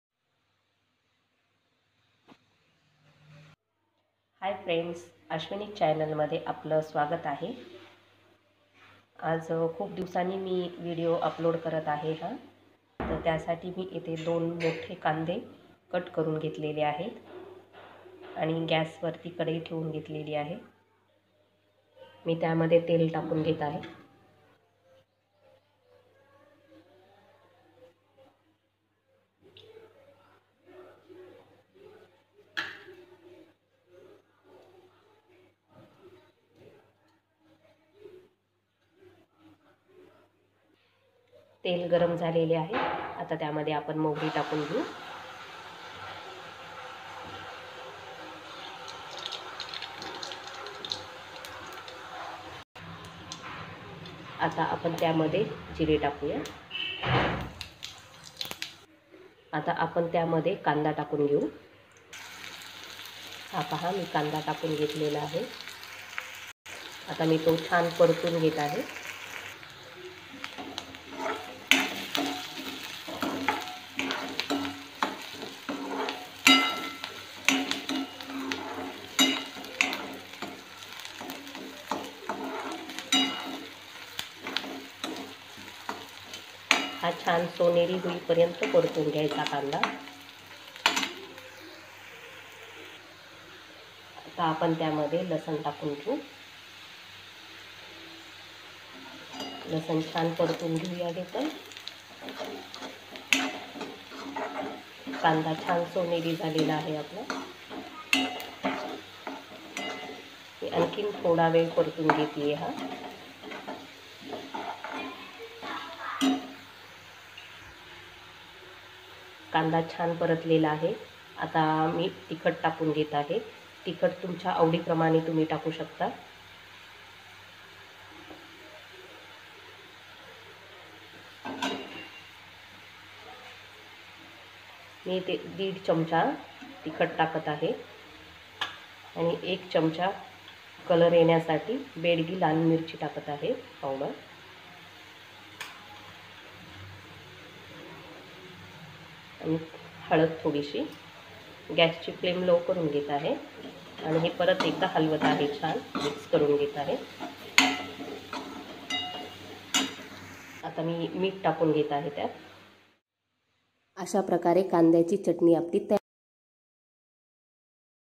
कांद्याची चटणी
कांद्याच्या चटणीसाठी लागणारे साहित्य:-
1 पुर्ण कांदा
आवडी प्रमाणे तिखट,मीठ